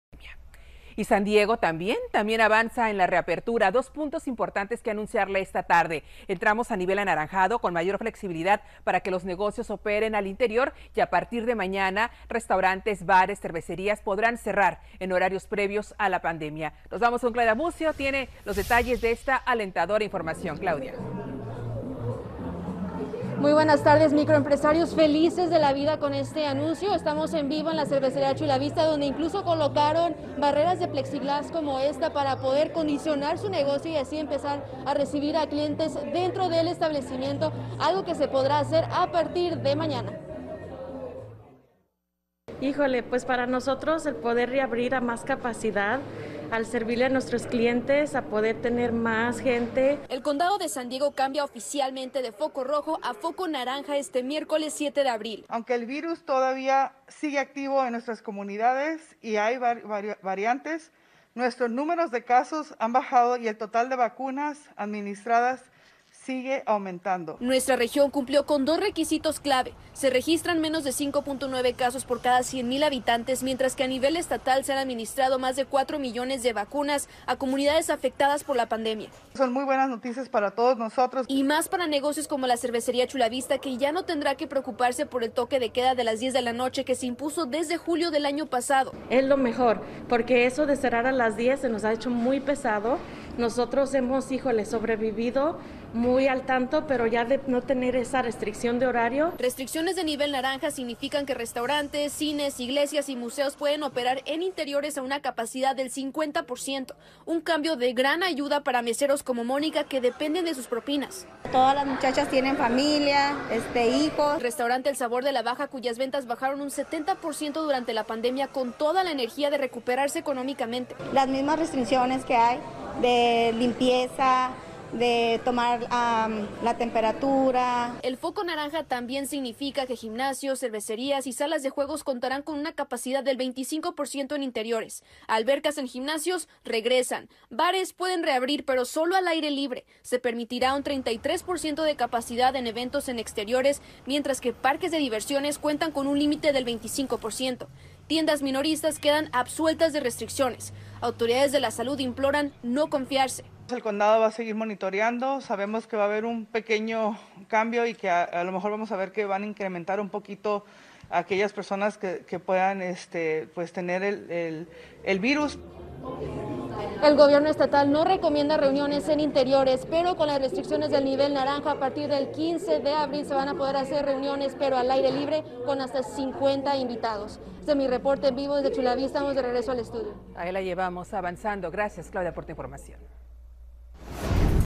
San Diego cambia a foco naranja con menos restricciones
El condado de San Diego pasa al foco naranja o sea menos restricciones sobre todo para restaurantes, cervecerías y gimnasios.